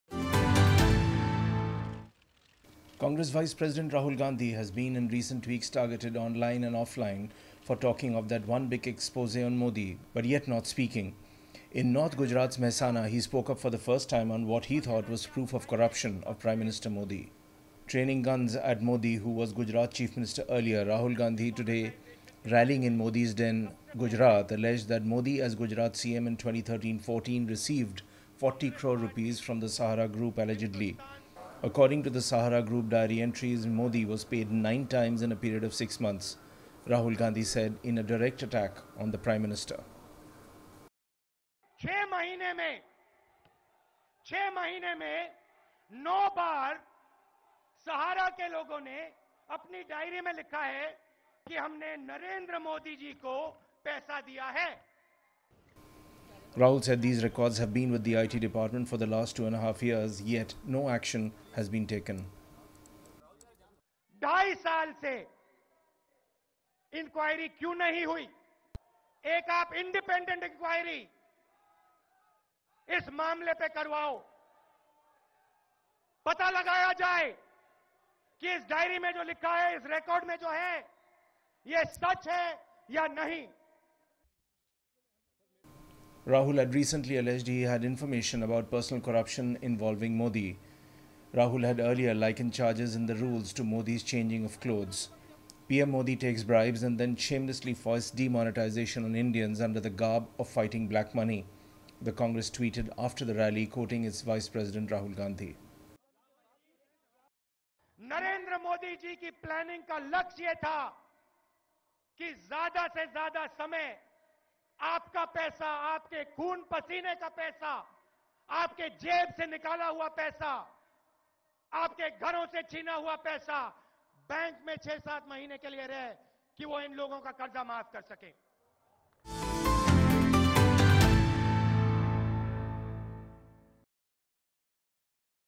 Rahul Gandhi Opens His Cards In Modi’s Den Gujarat
Congress Vice President, Rahul Gandhi has been in the recent weeks targetted online and offline for talking of that one big expose on Modi but yet not speaking. In North Gujarat’s Mehsana he spoke up for the first time on what he thought was proof of corruption of PM Modi